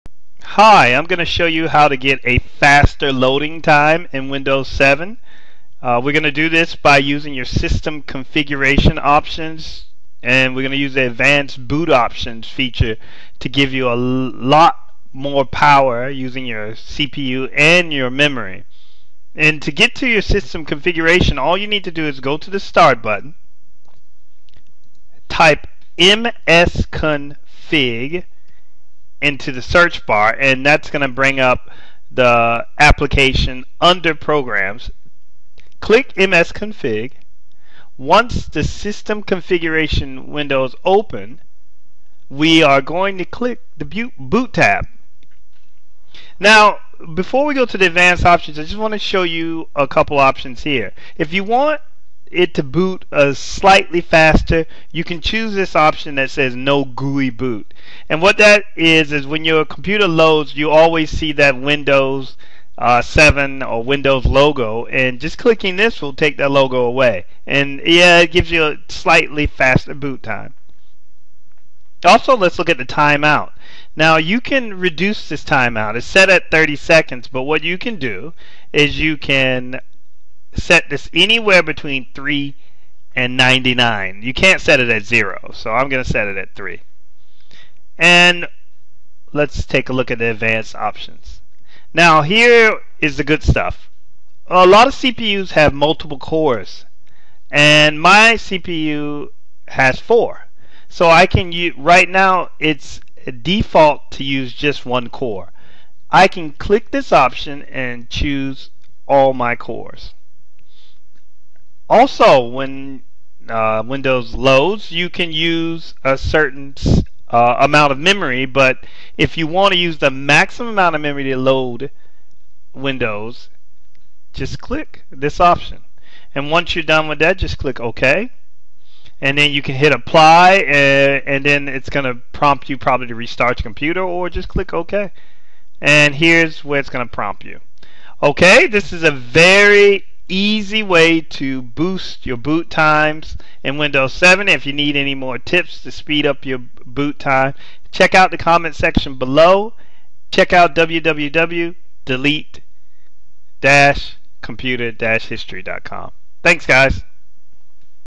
Windows Loads Slow | How to Speed Up Windows 7 Startup
To Speed Up Windows 7:
What to to do when Windows Loads Slow? You can use Windows 7 Booting option to speed up booting time easily.

Using the advanced Boot Options can reduce booting time in Windows considerable. You can access the Boot Options through the system configuration menu. Eliminating GUI boot, reducing Time Out options and  maxing out the memory are good was to speed up Windows 7 boot time.